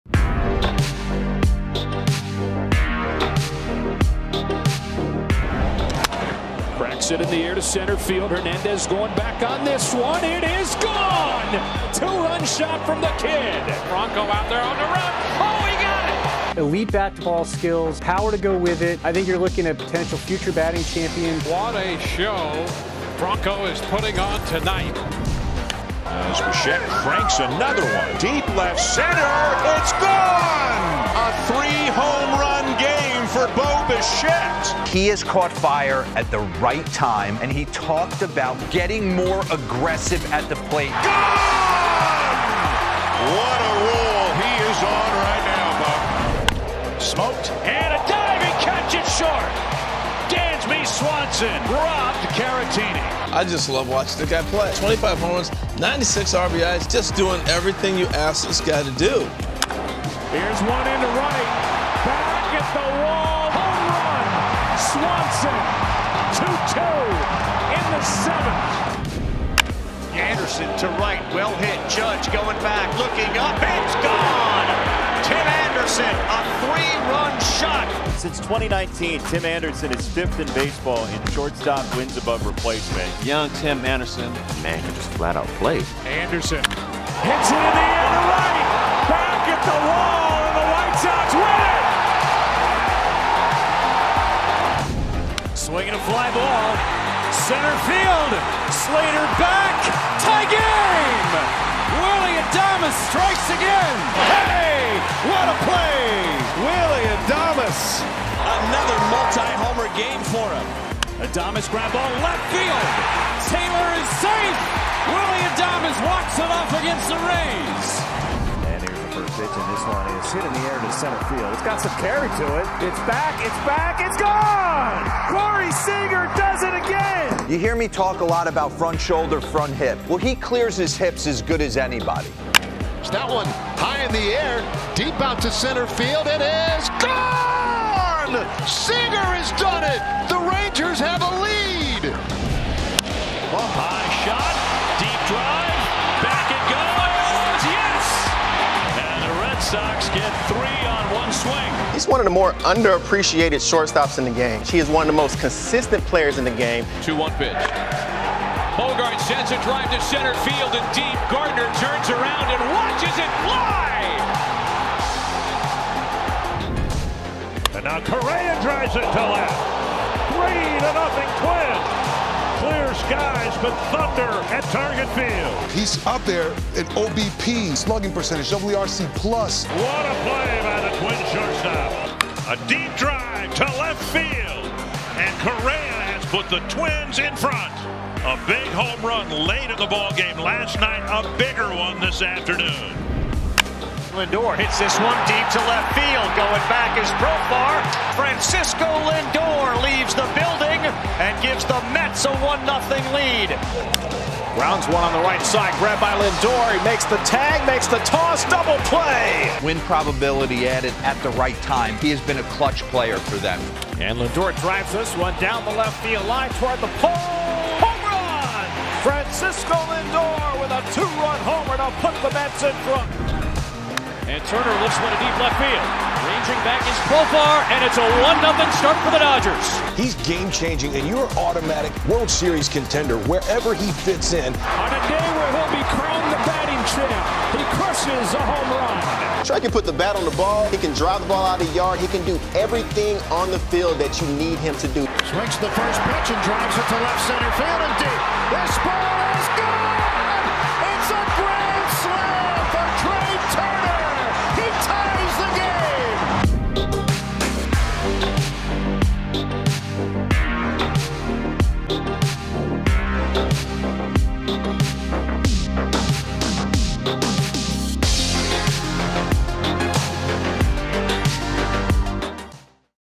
Top 10 Shortstops In MLB - 2023
We're back with another ranking video, this time we're talking about the studs up the middle, with our Top 10 Shortstops going into 2023 (Projection Based). There's plenty of talent at this position and this was a tough ranking to make, please leave your Top 10 in the comments below! Enjoy!

Check out the podcast to hear more about why we ranked these players how we did

Check out HalfStHighHeat.com for more content!

Writers: Allison - @JuanSotoHR CK - @CKNaim58 Danny - @Nats_Baseball Matt - @HolleranMDH Tyler - @nonrosterinvite @thezandstorm